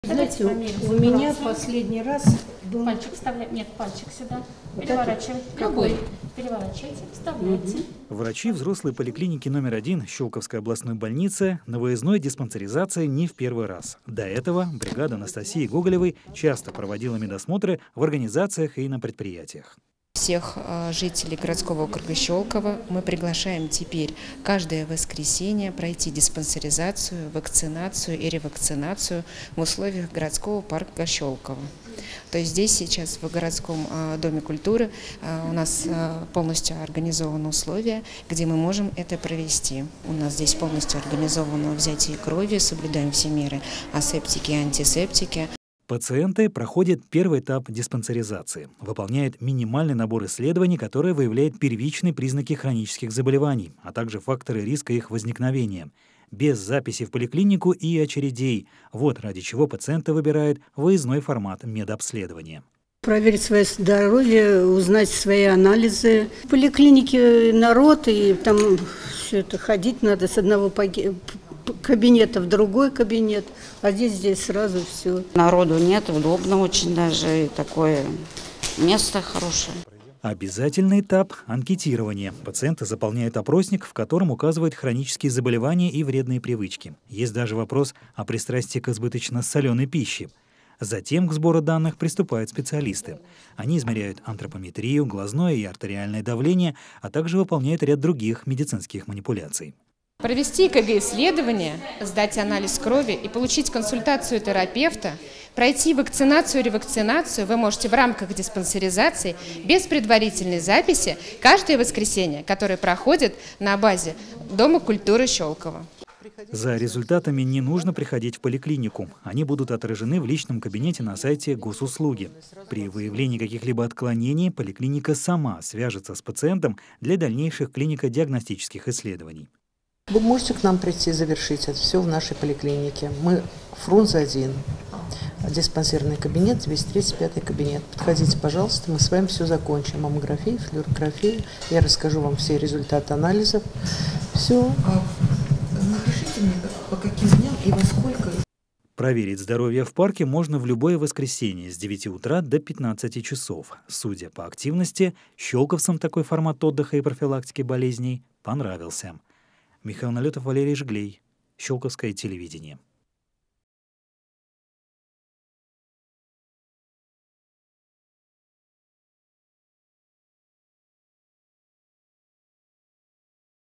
Бесплатная диспансеризация в Щёлковском центральном парке
В Щёлковском парке культуры и отдыха теперь можно бесплатно пройти скрининг на выявление хронических заболеваний, признаки которых едва заметы. Вместе с первыми пациентами в парк отравилась и наша съёмочная группа.